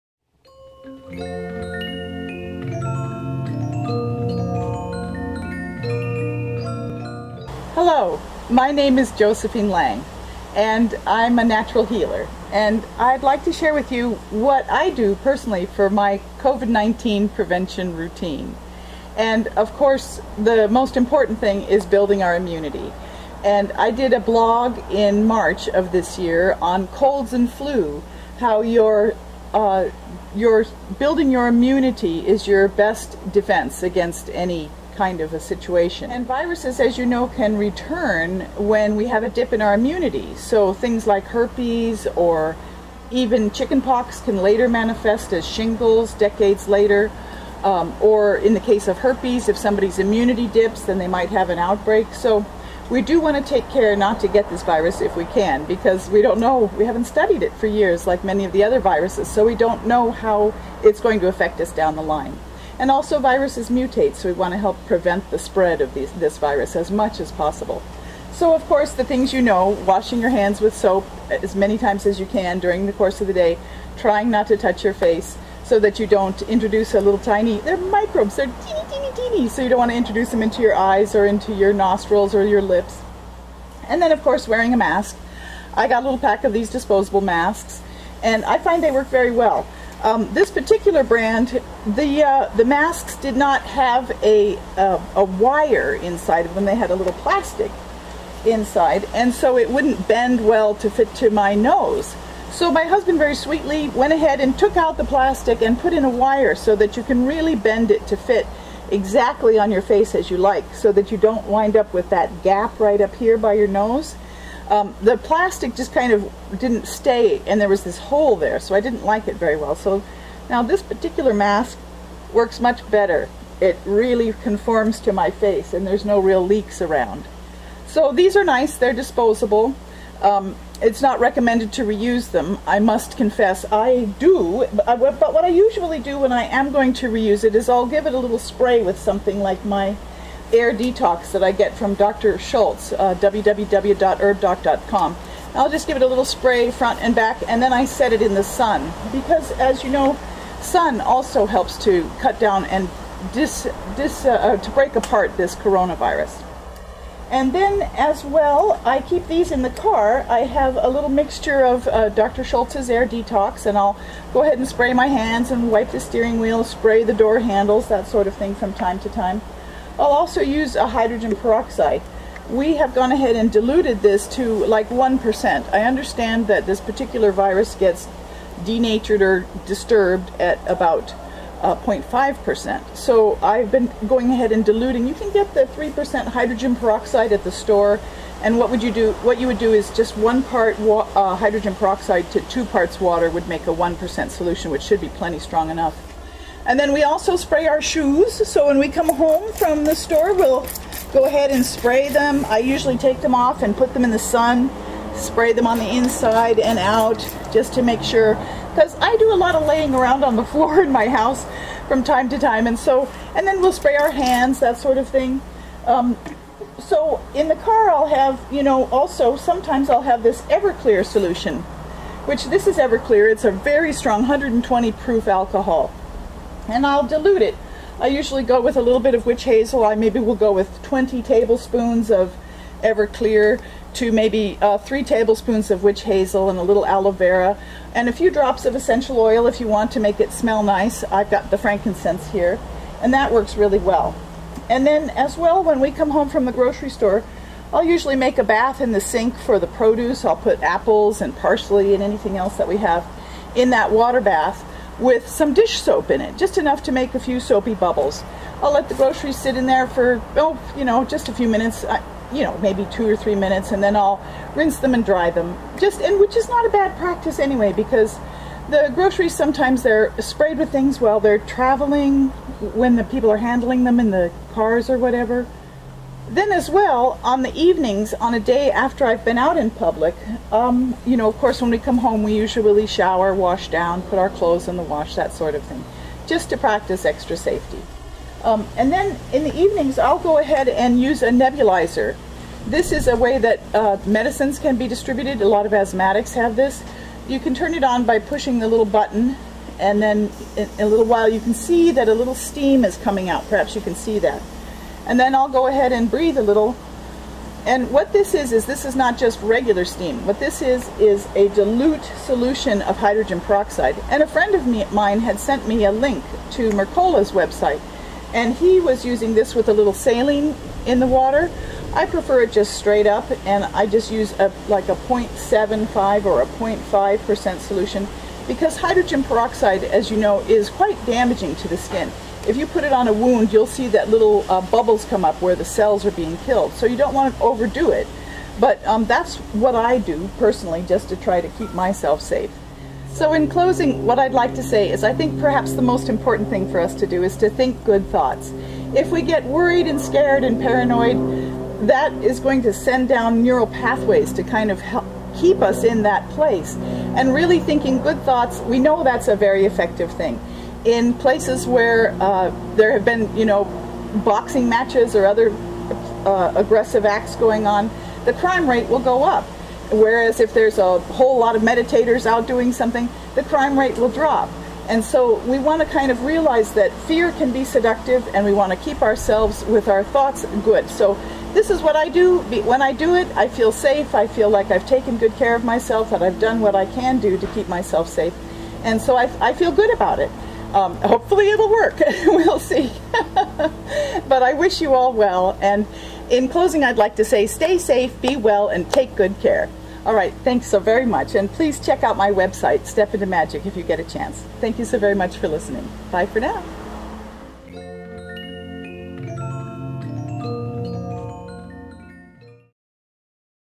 My COVID-19 Prevention Routine
Prevention, through a strong immune system, is always our best defense.  Washing hands and keeping our fingers away from our face, we already know.  Helping to mitigate the spread of this virus by minimizing our release of and contact with the droplets from "mouth rain," is also a good idea.  Simple sanitizing sprays or washing items with soap can be beneficial as well.  Hydrogen peroxide, (best to start with 30% as it has no stabilizing additives,) can be diluted to less than 1% and used in a nebulizer.  It creates a steam-like cool mist which can be inhaled.  I use it on days when I may have been exposed to lots of people or possible carriers of the virus, to help clear my sinuses and lungs.  (See Dr. Mercola's website for exact details on healing with h2o2.)  Take good care of yourself and know that if you do, you are going to be safe.  Then you can hold good thoughts and be well.  And, please enjoy this 7 minute video.

Also, please see my disclaimer on the "About" page of my YouTube Channel before viewing this video.